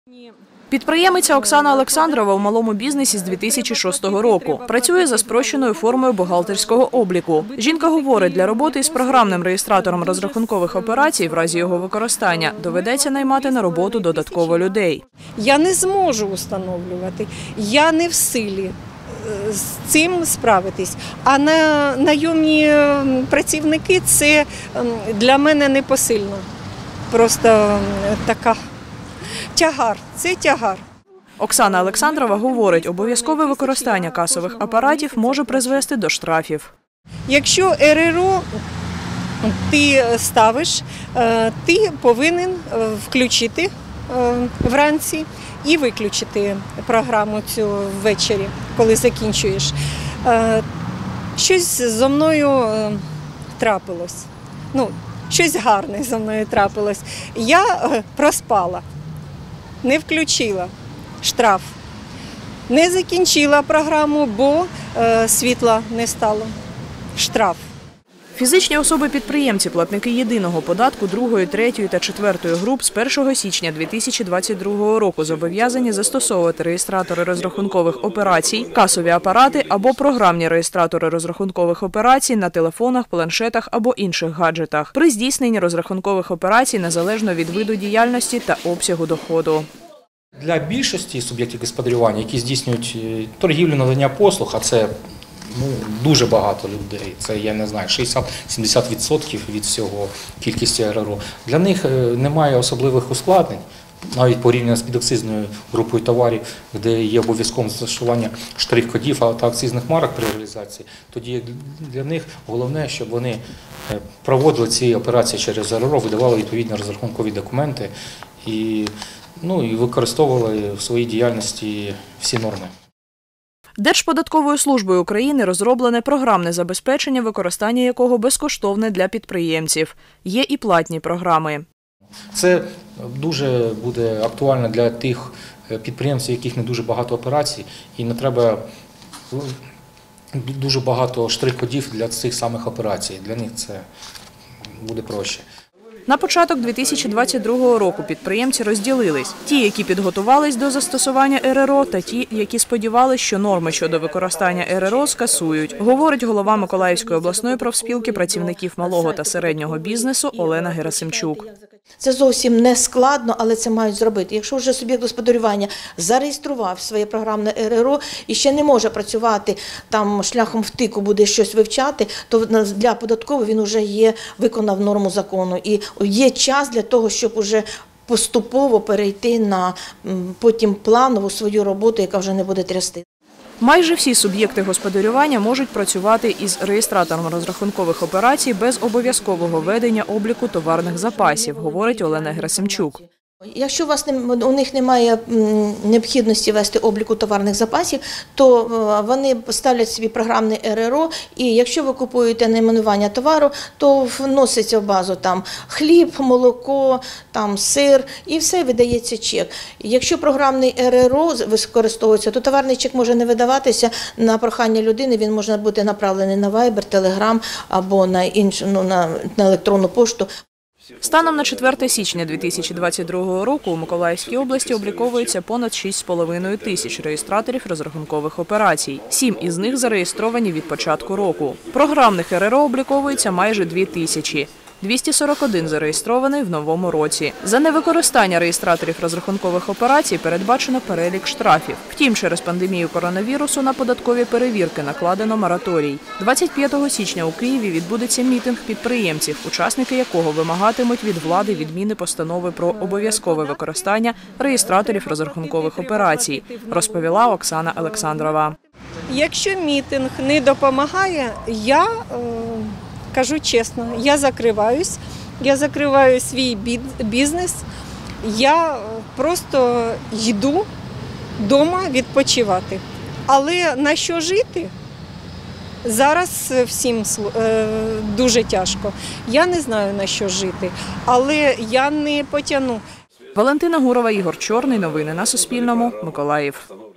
На Миколаївщині підприємці встановлюють реєстратори розрахункових операцій
Від початку 2022 року на Миколаївщині підприємці встановили майже 250 реєстраторів розрахункових операцій, серед яких 241 програмний.

Авторка - Валентина Гурова, оператор - Ігор Чорний.